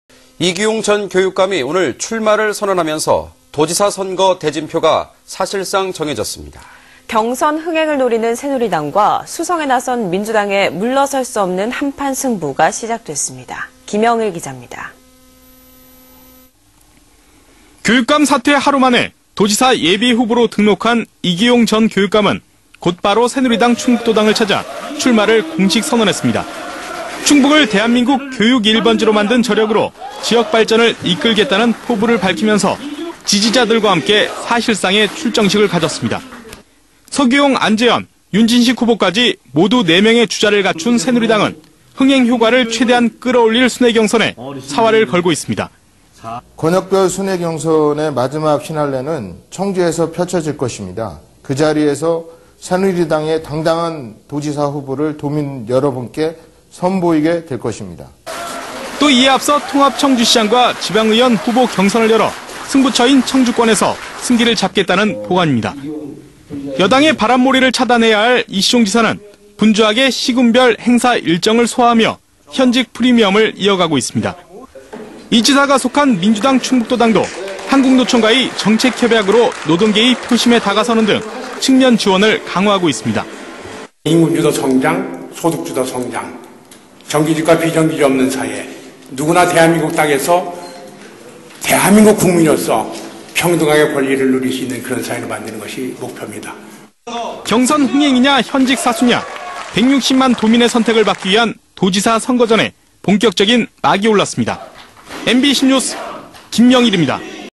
[청주MBC뉴스]경선 흥행 vs 현직 사수
◀ＡＮＣ▶
이기용 전 교육감이 오늘(6) 출마를
선언하면서, 도지사 선거 대진표가 사실상
정해졌습니다. 경선 흥행을 노리는 새누리당과
수성에 나선 민주당의 물러설 수 없는
한판 승부가 시작됐습니다. 김영일 기자입니다.

◀ＶＣＲ▶
교육감 사퇴 하루 만에 도지사 예비후보로
등록한 이기용 전 교육감은 곧바로 새누리당
충북도당을 찾아 출마를 공식 선언했습니다.

충북을 대한민국 교육1번지로 만든 저력으로
지역 발전을 이끌겠다는 포부를 밝히면서
지지자들과 함께 사실상의 출정식을 가졌습니다

서규용, 안재헌, 윤진식 후보까지 모두 4명의
주자를 갖춘 새누리당은 흥행 효과를 최대한
끌어올릴 순회 경선에 사활을 걸고 있습니다.

◀ＩＮＴ▶
이규석/새누리당 충북도당 사무처장
(경선 피날레는 청주에서 펼쳐질 것)

또, 이에 앞서 통합청주시장과
지방의원 후보 경선을 열어 승부처인
청주권에서 승기를 잡겠다는 복안입니다.

여당의 바람몰이를 차단해야 할 이시종 지사는
분주하게 시군별 행사 일정을 소화하며
현직 프리미엄을 이어가고 있습니다.

이 지사가 속한 민주당 충북도당도
한국노총과의 정책협약으로 노동계의 표심에
다가서는 등 측면 지원을 강화하고 있습니다.

◀ＳＹＮ▶
변재일/민주당 충북도당위원장

경선 흥행이냐 현직 사수냐,
160만 도민의 선택을 받기 위한
도지사 선거전의 본격적인 막이 올랐습니다.
MBC NEWS 김영일입니다.
◀ＥＮＤ▶